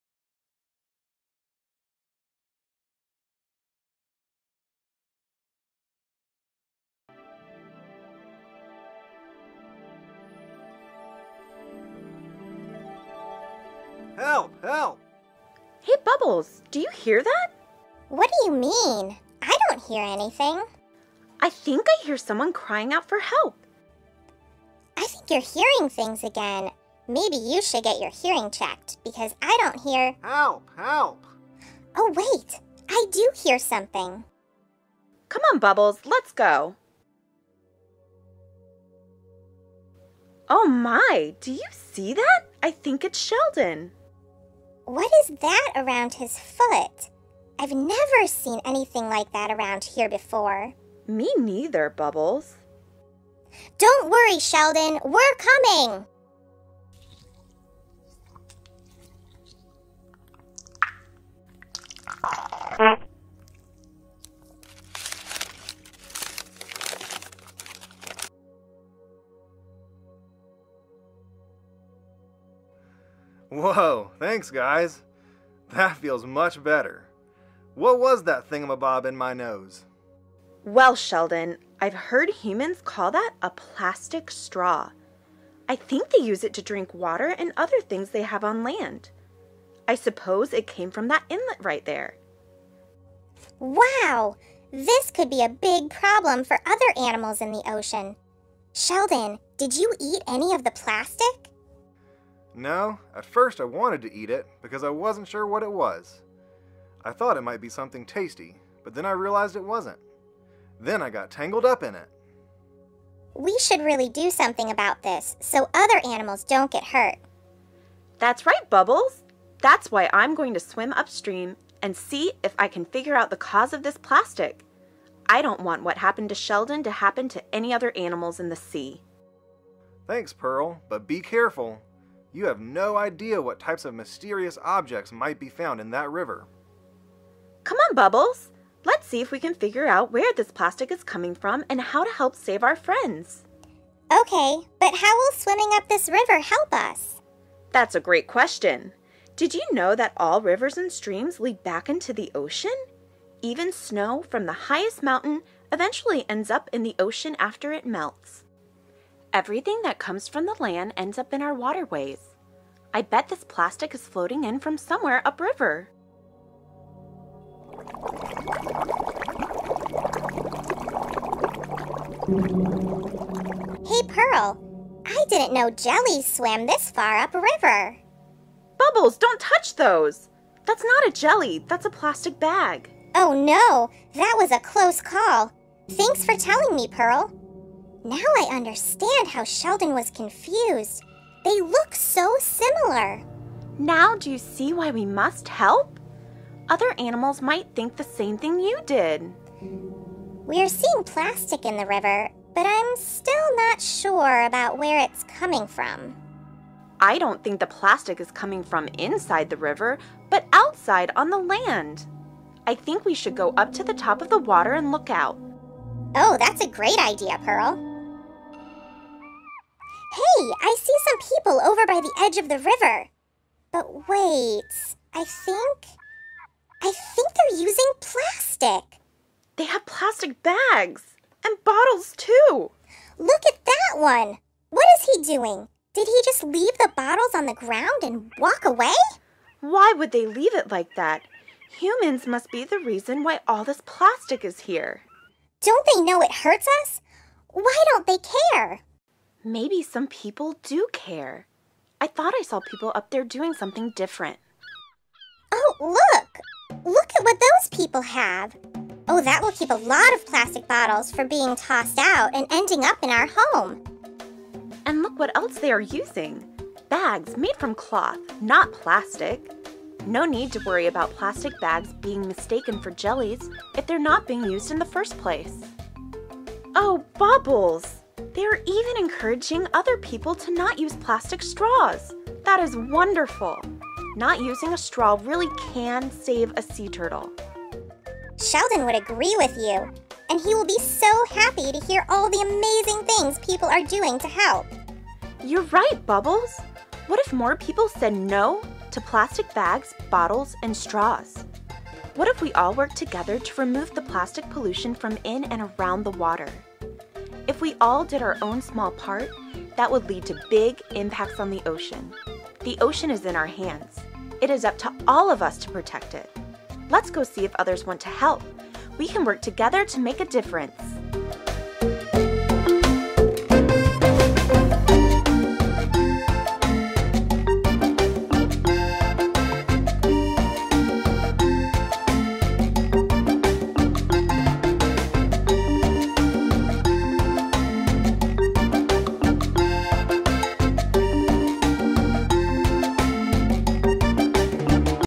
Mermaid Pearl's Plastic Problem
Join Pearl, the mermaid, and Bubbles, the fish, as they search for the source of plastic pollution found in their ocean home. The North Carolina Aquarium at Fort Fisher cares about the health of our ocean. Join our efforts by limiting the amount of single-use plastic items you use.